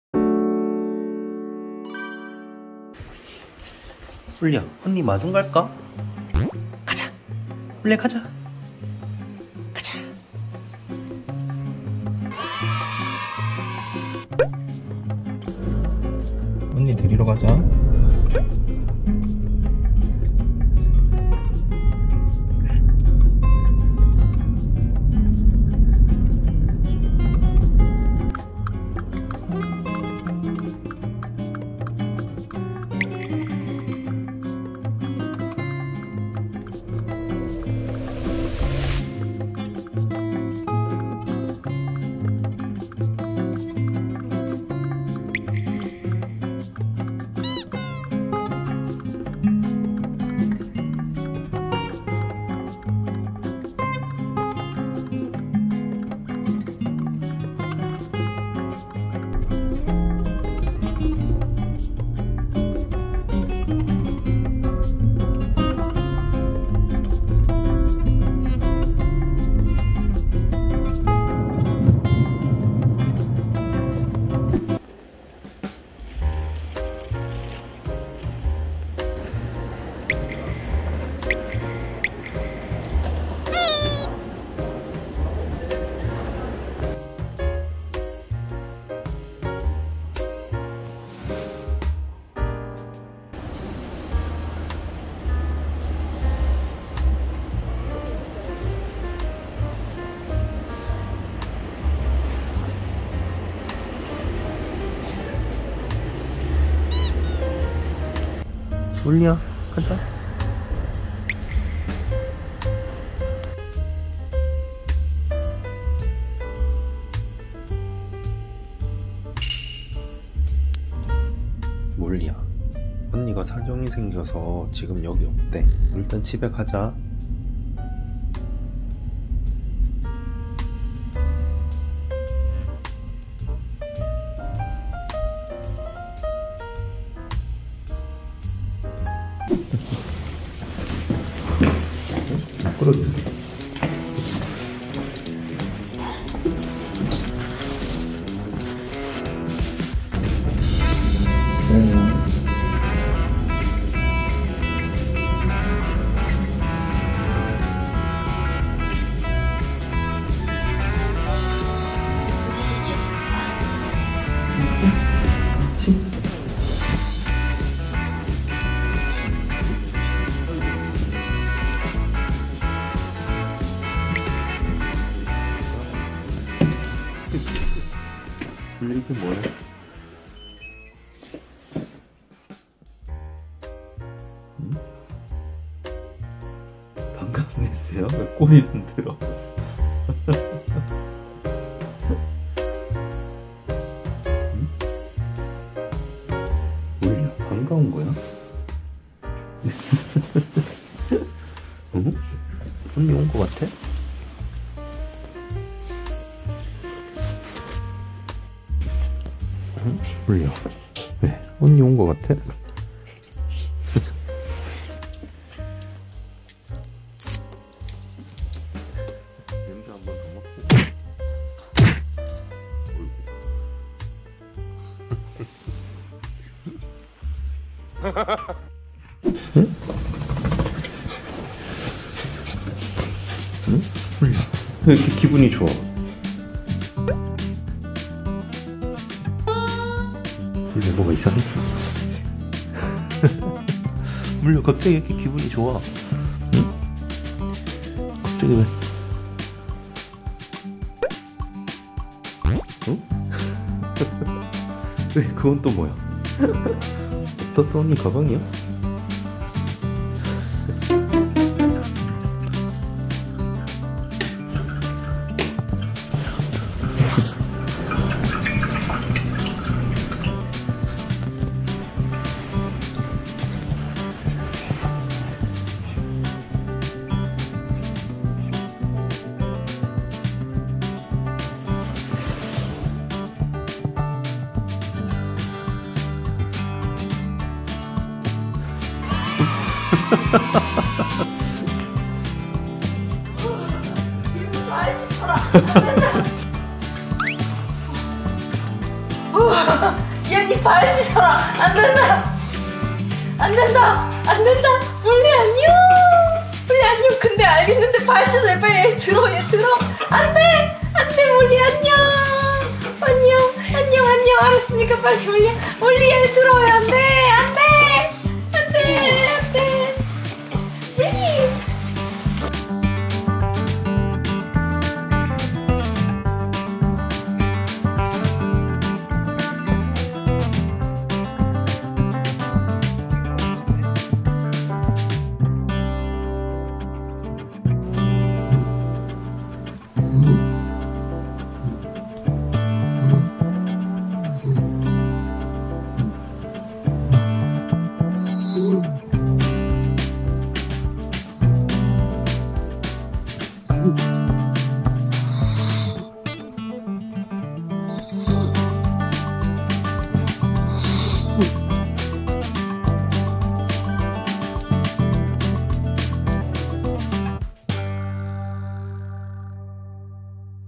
ENG | 출장 갔다 돌아온 언니를 만난 강아지의 반응(감동주의ㅠㅠ)
항상 언니 오빠 반겨주고 좋아해주는 귀여운 몰리🥰  많이 예뻐해주세요🙏🏻

댕댕이 몰리의 프로필
 이름 : 몰리🐶
 생년월일 : 2018. 06🎂
 성별 : 여 ♀︎
 특징: 새 쥬인은 필요 없다고 생각하는 강아지
 별명 : 공주, 잘생긴강아지😭, 언니바라기
 산책시 많이 듣는 말 : 참 순하네, 참 얌전하네, 참 잘생겼네..😭